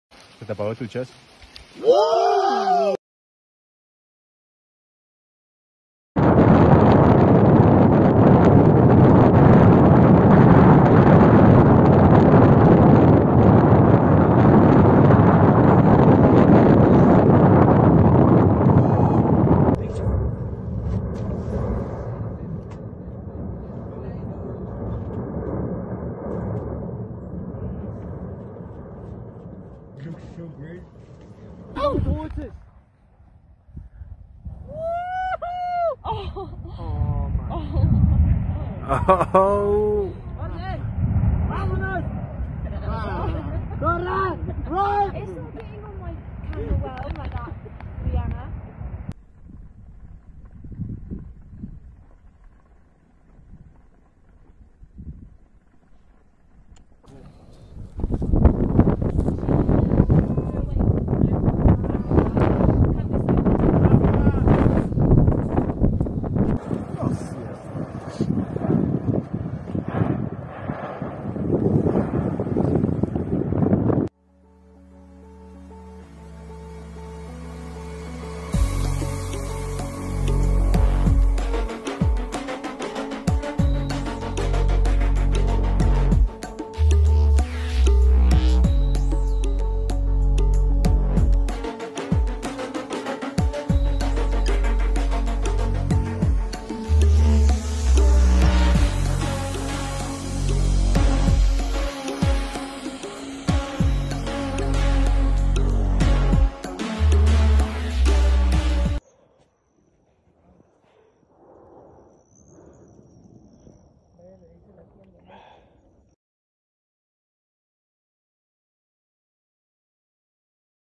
In Guatemala, the Fuego volcano erupted.
The eruption of the volcano Fuego, which is located in the central part of Guatemala, is accompanied by the eruption of ash and pyroclastic flows.